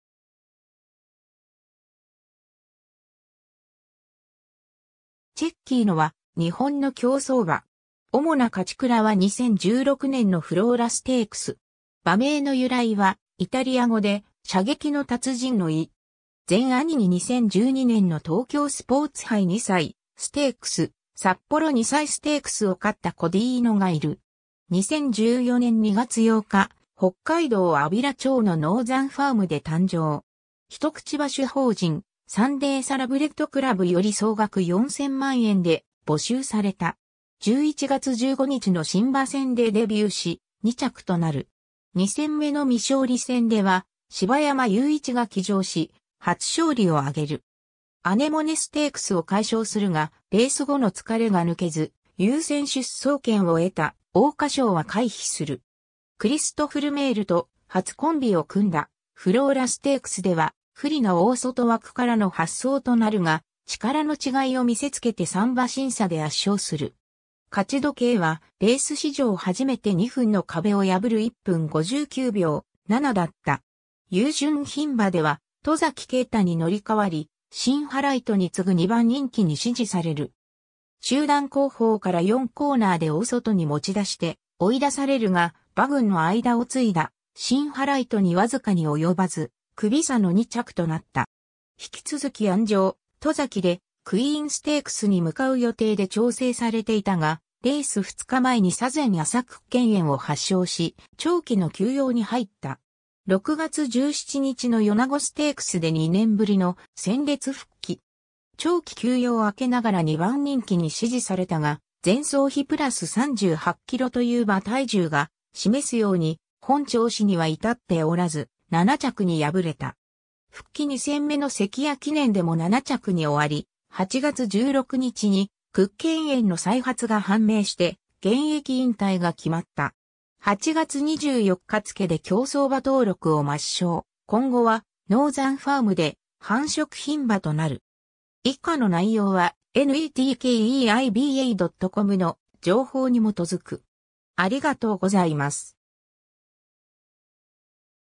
チェッキーノ
チェッキーノ（伊：Cecchino）は、日本の競走馬。
主な勝ち鞍は2016年のフローラステークス(GII)。
馬名の由来は「イタリア語で『射撃の達人』の意」。
 全兄に2012年の東京スポーツ杯2歳ステークス(GIII)、札幌2歳ステークス(GIII)を勝ったコディーノがいる。
 2014年2月8日、北海道安平町のノーザンファームで誕生。
一口馬主法人「サンデーサラブレッドクラブ」より総額4,000万円（1口100万円×40口）で募集された。
 11月15日の新馬戦でデビューし、2着となる。
2戦目の未勝利戦では柴山雄一が騎乗し、初勝利を挙げる。
 アネモネステークス(OP)を快勝するが、レース後の疲れが抜けず、優先出走権を得た桜花賞(GI)は回避する。
クリストフ・ルメールと初コンビを組んだフローラステークス(GII)では不利な大外枠からの発走となるが、力の違いを見せつけて3馬身差で圧勝する。
勝ち時計はレース史上初めて2分の壁を破る1分59秒7だった。
 優駿牝馬(GI)では戸崎圭太に乗り替わり、シンハライトに次ぐ2番人気に支持される。
中団後方から4コーナーで大外に持ち出して追い出されるが、馬群の間を突いだシンハライトに僅かに及ばず、クビ差の2着となった。
 引き続き鞍上・戸崎でクイーンステークス(GIII)に向かう予定で調整されていたが、レース2日前に左前浅屈腱炎を発症し、長期の休養に入った。
 6月17日の米子ステークス(OP)で2年ぶりの戦列復帰。
長期休養明けながら2番人気に支持されたが、前走比プラス38キロという馬体重が示すように本調子には至っておらず、7着に敗れた。
復帰2戦目の関屋記念でも7着に終わり、8月16日に屈腱炎の再発が判明して現役引退が決まった。
8月24日付で競走馬登録を抹消、今後はノーザンファームで繁殖牝馬となる。